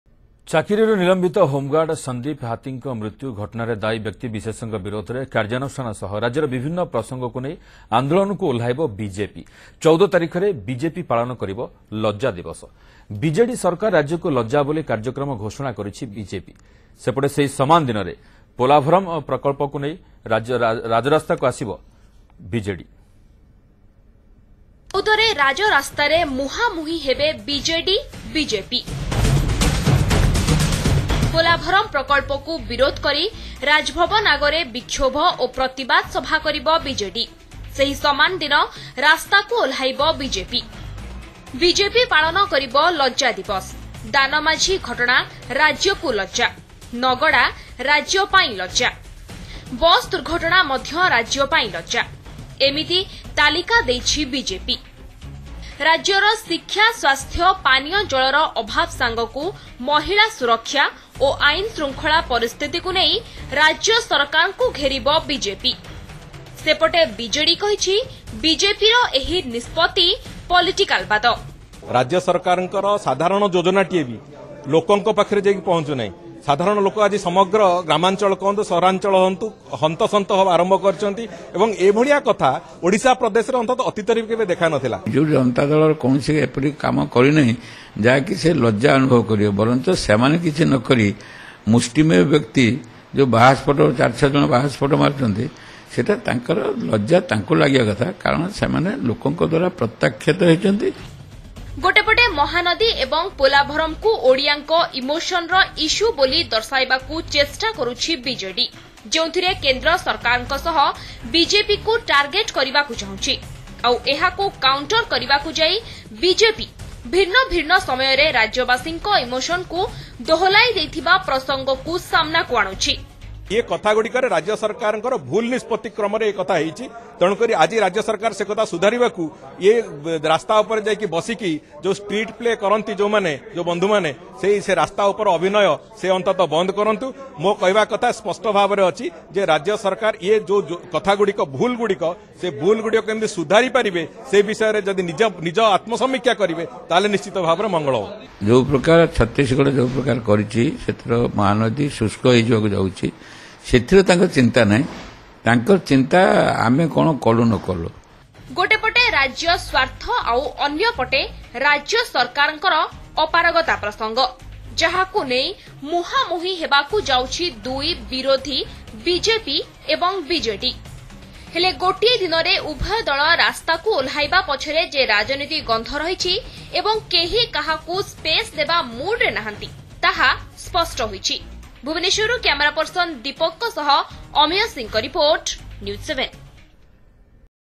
BJP to observe Sep 14 as Lajja Diwas: prime time odisha
The State BJP unit will observe September 14 as ‘Lajja Diwas’ in connection with the death of expelled Home Guard Sandeep Hati.